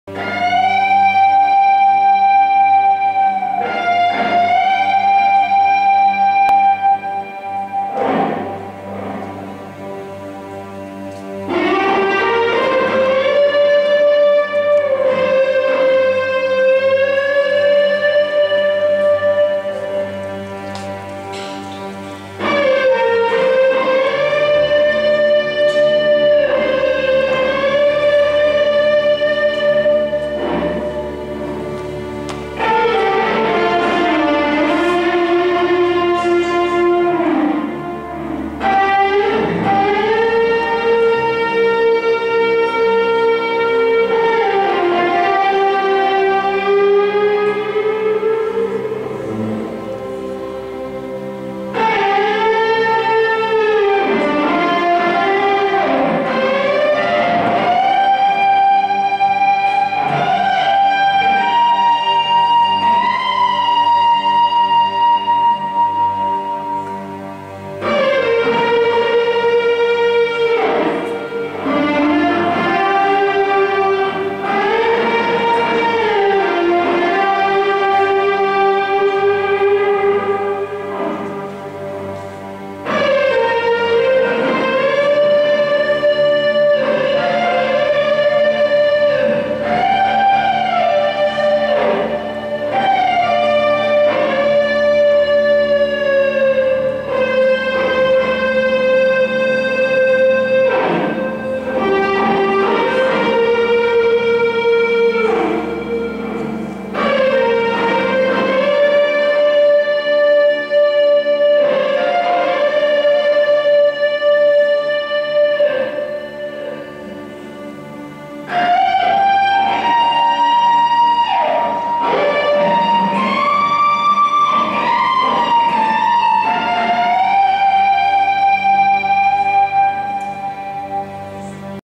Orchestra Filarmonica Valente, Elfo Puccini - Milano - 18 maggio 2017
Esibizione dell'Orchestra Filarmonica Valente presso il Teatro ELFO PUCCINI in Milano,
dirigono: Salvatore VALENTE e Giuseppe GUIDA
18 Maggio 2017
prove, concerto, clips